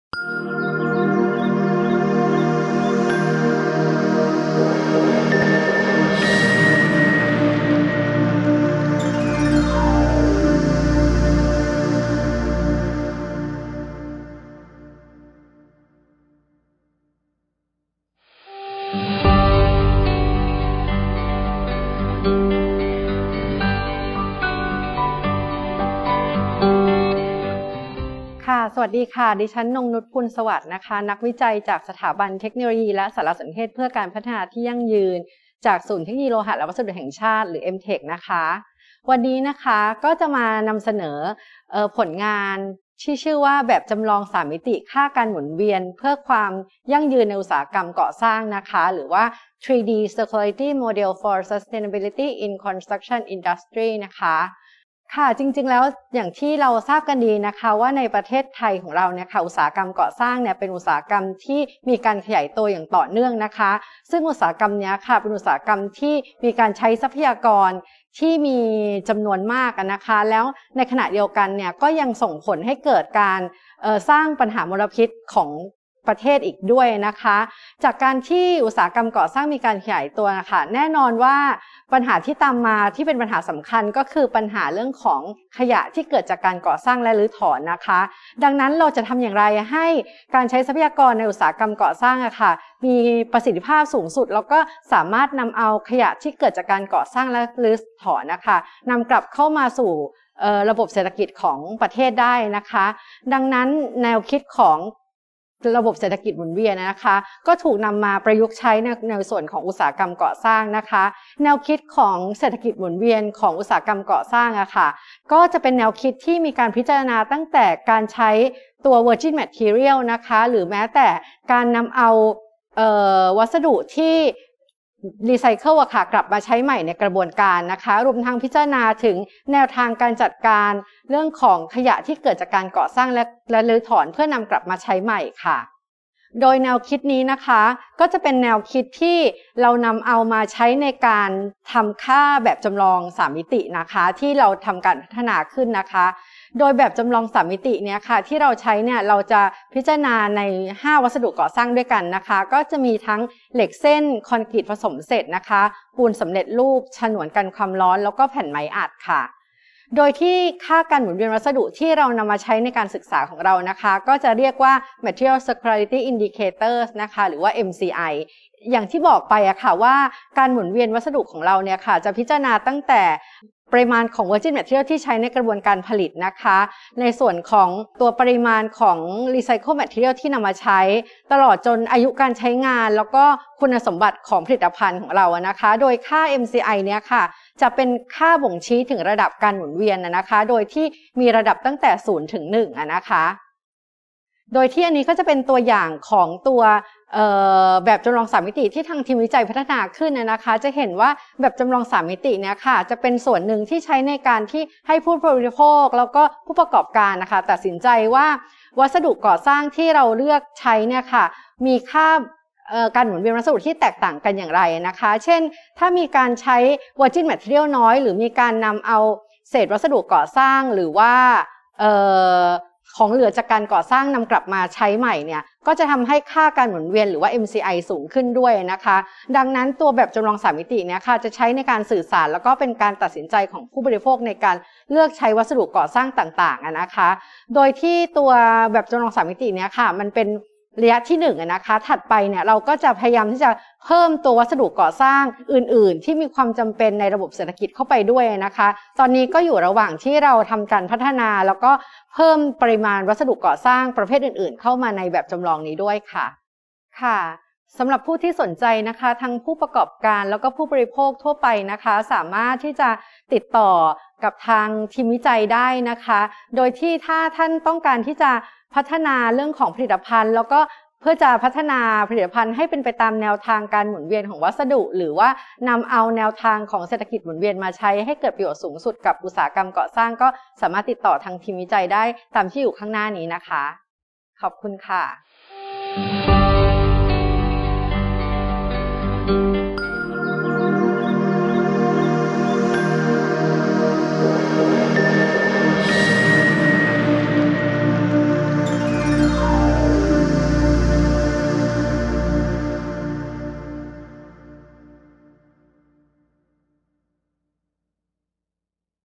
แบบจำลองสามมิติการหมุนเวียน เพื่อความยั่งยืนในอุตสาหกรรมก่อสร้าง
อุตสาหกรรมก่อสร้างมีการใช้ทรัพยากรจำนวนมาก ก่อให้เกิดปัญหาขยะที่เกิดจากการก่อสร้างและรื้อถอนส่งผลทำให้เกิดมลพิษต่อประเทศ แนวคิดการหมุนเวียนของวัสดุก่อสร้างถูกนำมาประยุกต์ใช้โดยนำมาทำค่าแบบจำลองสามมิติ ซึ่งพิจารณาใน 5 วัสดุก่อสร้าง ได้แก่ เหล็กเส้น คอนกรีตผสมเสร็จ ปูนซีเมนต์สำเร็จรูป ฉนวนกันความร้อน และแผ่นไม้อัด โดยค่าการหมุนเวียนของวัสดุที่นำมาศึกษา เรียกว่า Material Circularity Indicator : MCI